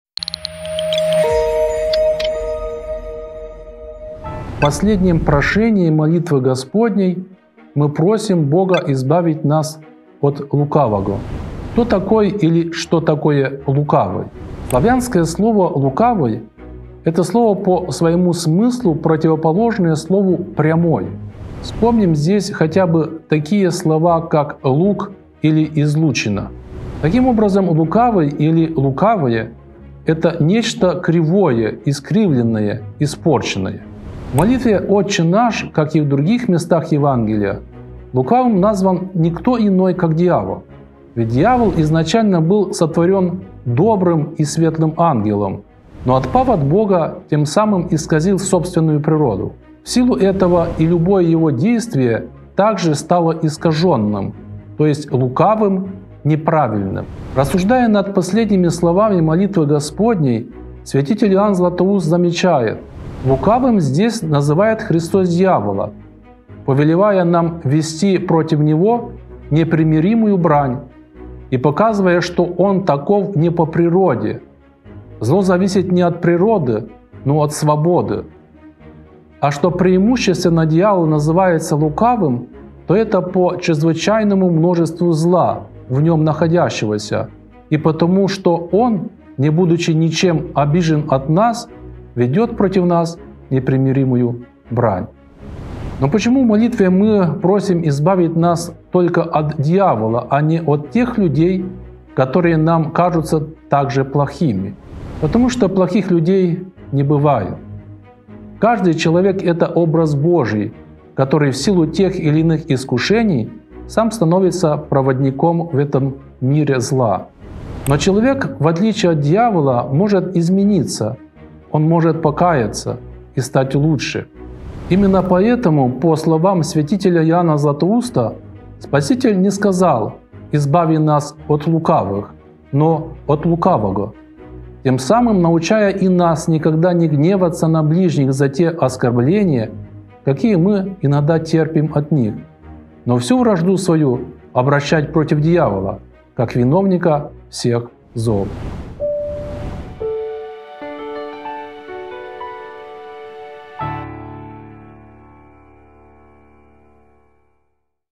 ОТЧЕ НАШ 8-я часть. Прошение: «Но избави нас от лукаваго». Митр. Антоний (Паканич)
Как можно любить ближних и при этом не прощать им их проступков по отношению к нам? Никак. Это невозможно. Именно поэтому мы и просим Бога, чтобы Он простил нам наши грехи, подобно тому, как и мы прощаем то, что сделано в наш адрес.